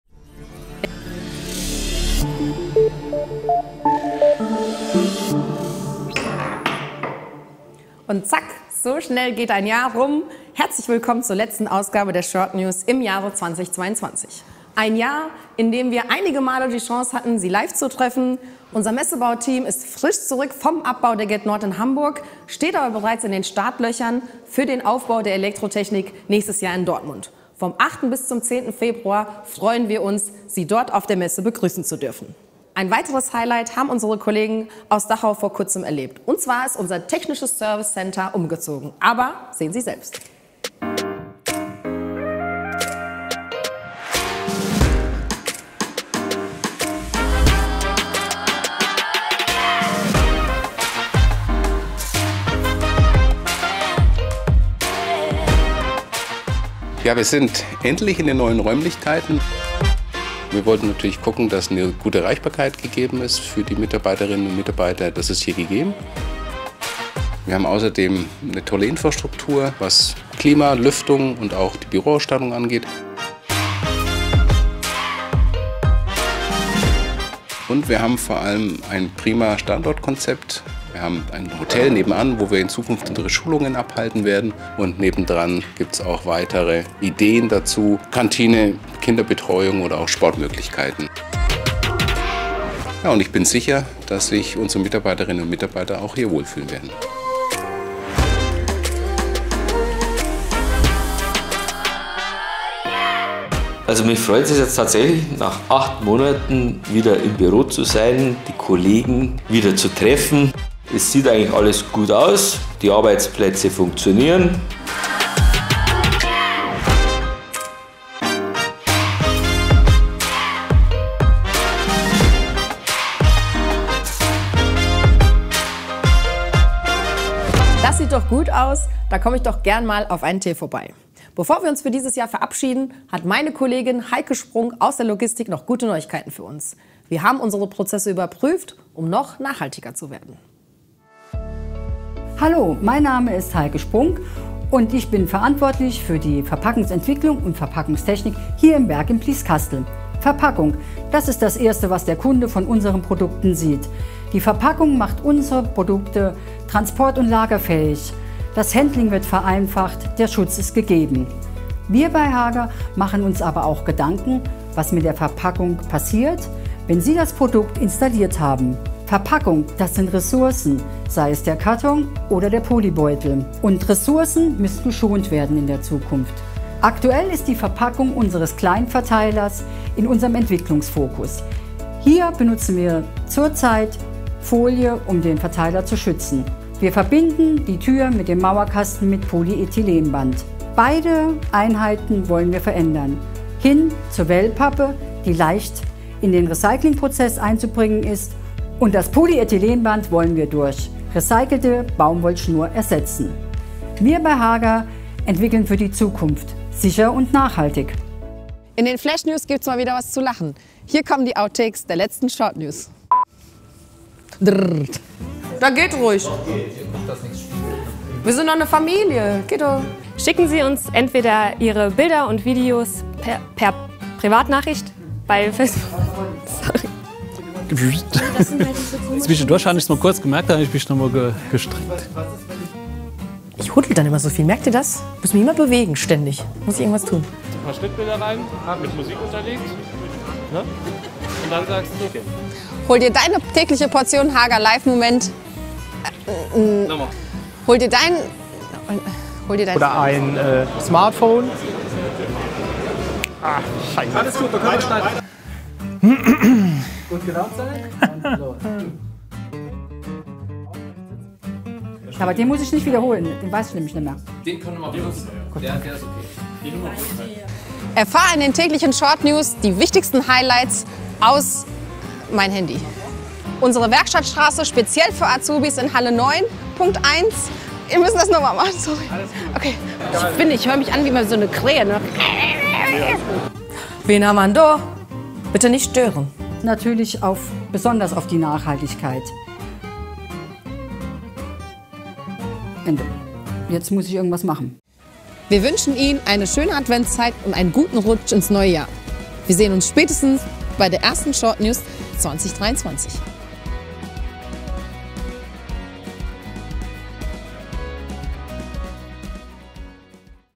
Hager shortnews #8: TSC München | Nachhaltigkeit in der Verpackung | Outtakes 2022
In der letzten shortnews des Jahres 2022 starten wir mit einen Blick in die neuen Räumlichkeiten des Technischen Service Centers München. Außerdem gibt's nachhaltige Neuigkeiten aus dem Bereich Verpackung. Am Ende folgen die Flashnews – diesmal mit den schönsten Outtakes aus den Shortnews in diesem Jahr.

00:00 Intro
00:10 Begrüßung
00:41 Technisches Service Center München umgezogen
02:21 So schonen Hager Verpackungen Ressourcen für die Zukunft
03:29 Flashnews: Outtakes 2022
04:55 Verabschiedung & Festtagsgrüße
05:08 Outro